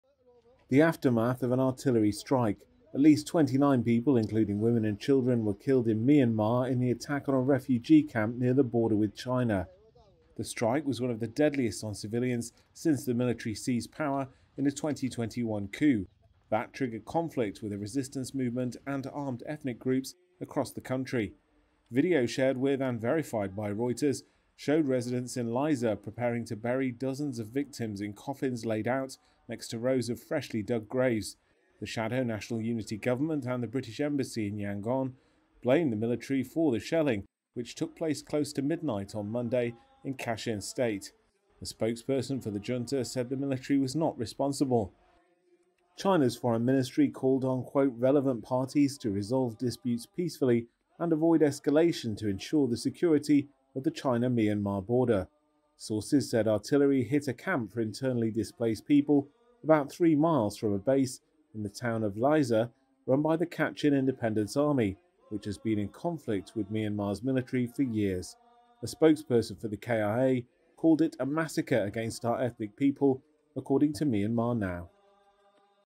Women, children killed at Myanmar refugee camp, sources say
The International Monetary Fund cut its growth forecasts for China and the euro zone and said overall global growth remained low and uneven despite what it called the 'remarkable strength' of the US economy.